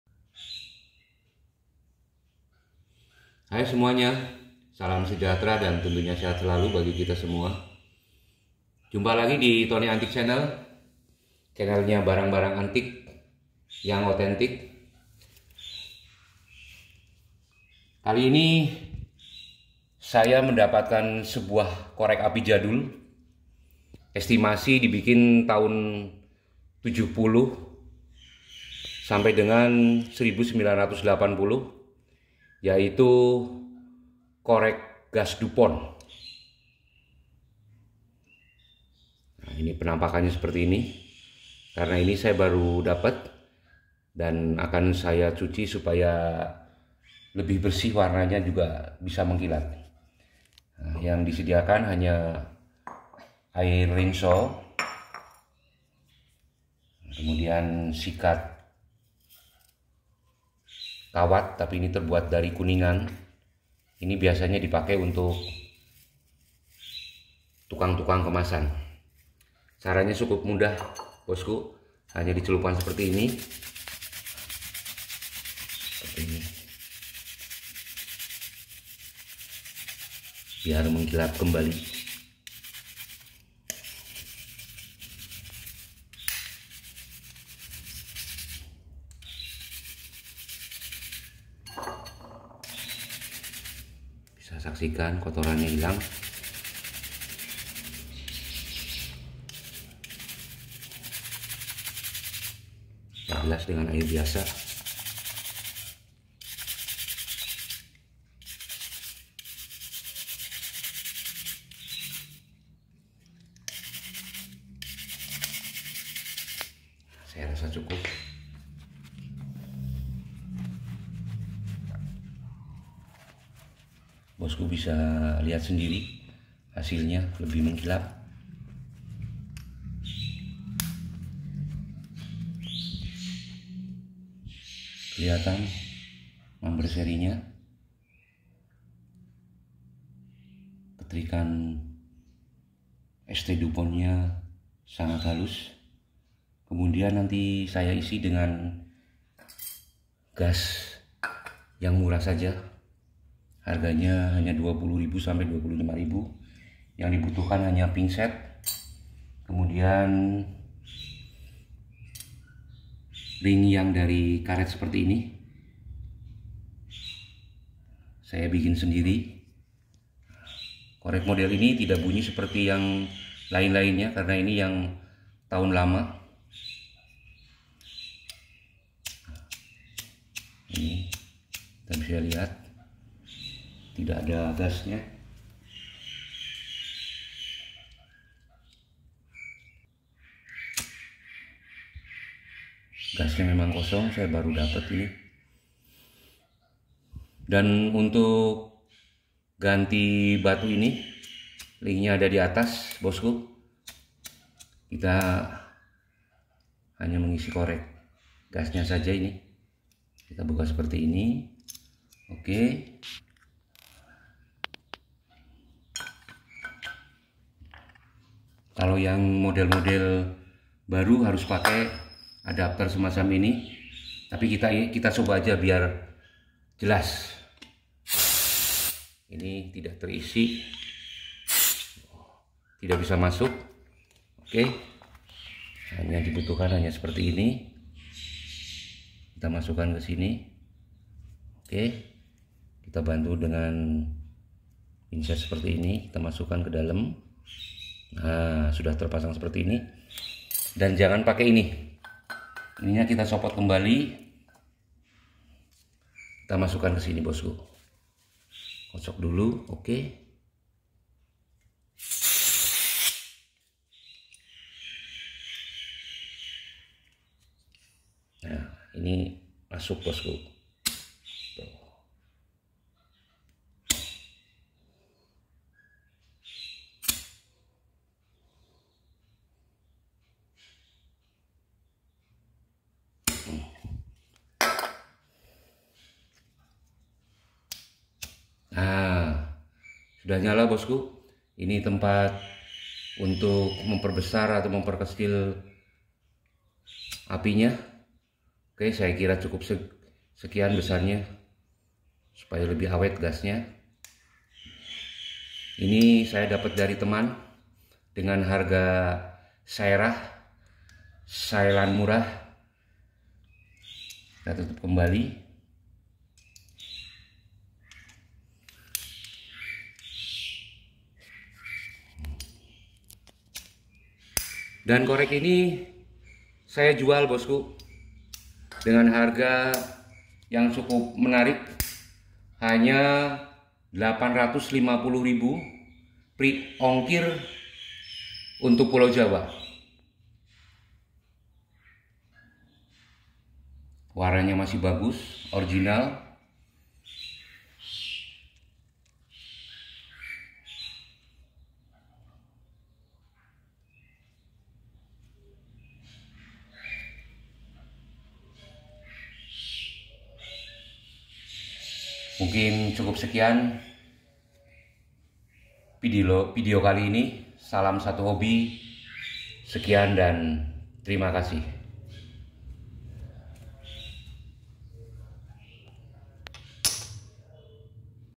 DIJUAL MURAH Korek Api ST Dupont Ligne Gold Finish Made in France | Paris Lighter
Miliki korek api ST Dupont Ligne Gold finish ini dengan harga super murah Boss Ku...
Walaupun ST Dupont merupakan salah satu brand mewah dan terkenal diseluruh dunia, tapi jangan khawatir dengan perawatan atau pengisian gas nya yang terkenal mahal. Disini saya berikan tutorial lengkap cara mengisi gas nya dengan cara yang sangat mudah, dan terpenting sangat HEMAT.

Teman teman tonton sampai akhir yaa...
Semoga bermanfaat 🙏🙏